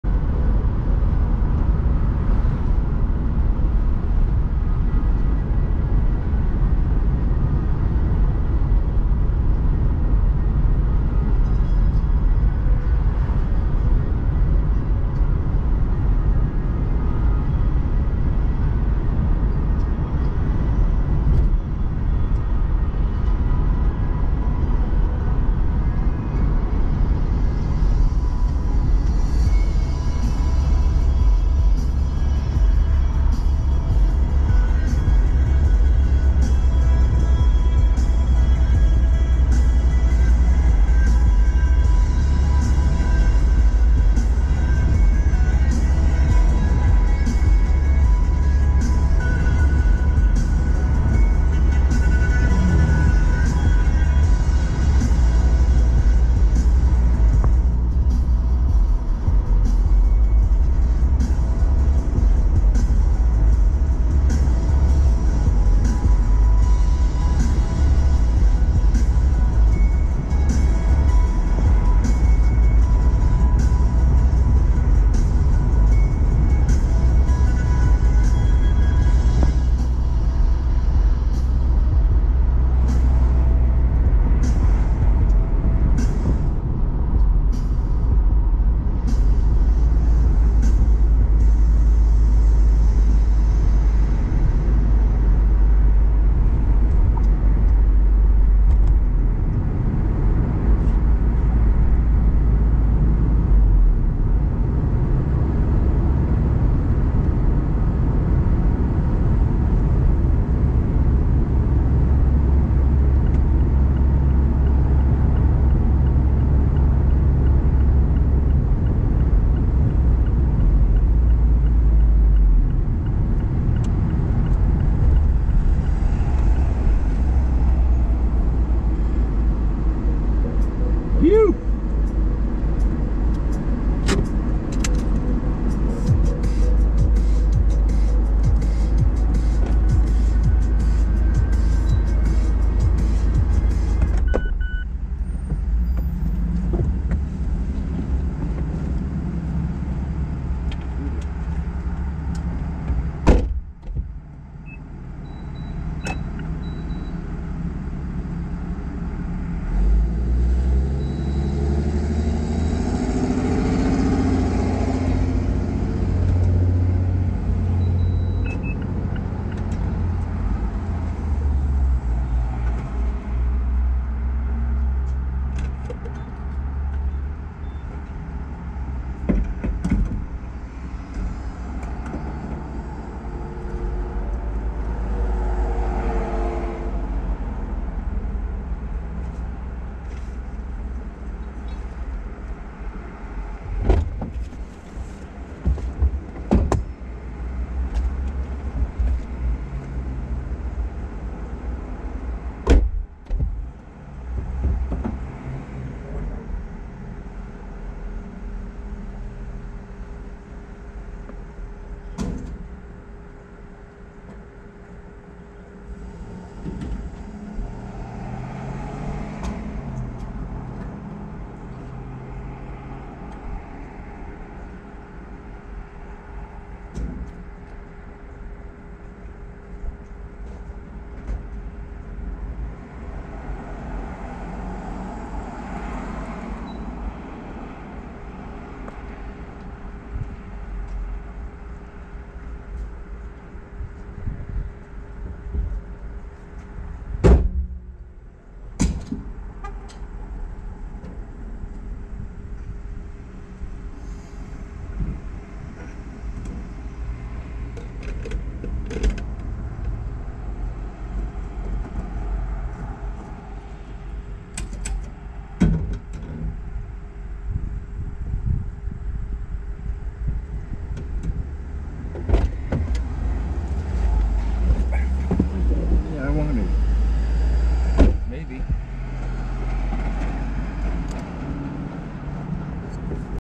Heading to get gas
I looked down and saw the gas light on. I was returning from a trackday at Club Motorsport in Tamworth ,NH. I switched the info button to tell me the range, it showed 0 miles left. I was still on the side road a few miles before I got to Rt 302. I knew I still had a few miles to get into town to fill-up at the Circle K. That's why you hear a whew as I pull in.